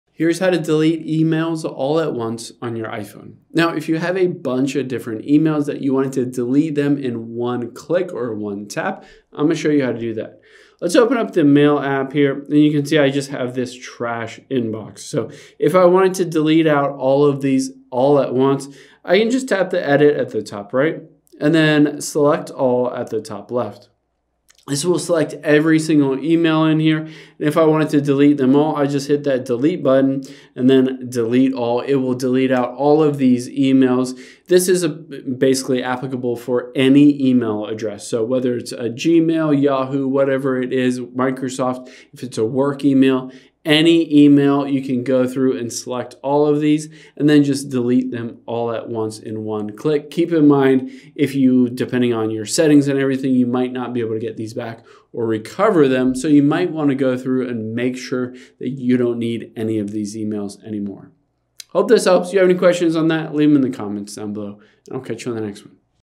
How To Delete Emails All At Once On iPhone
Let's delete all of your emails all at once on your iPhone if you have hundreds or thousands of emails you need to remove.